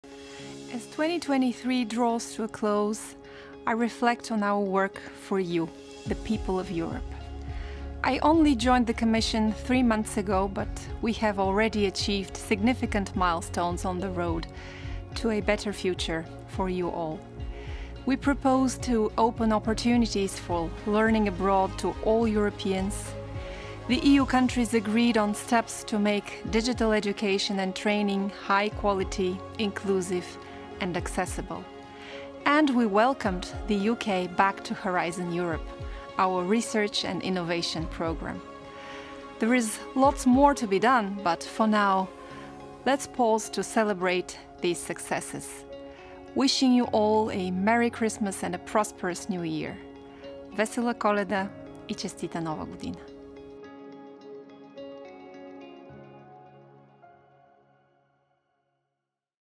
Digital education and high quality, inclusive and accessible training is a reality in the EU
Iliana Ivanova, European Commissioner for Innovation, Research, Culture, Education and Youth.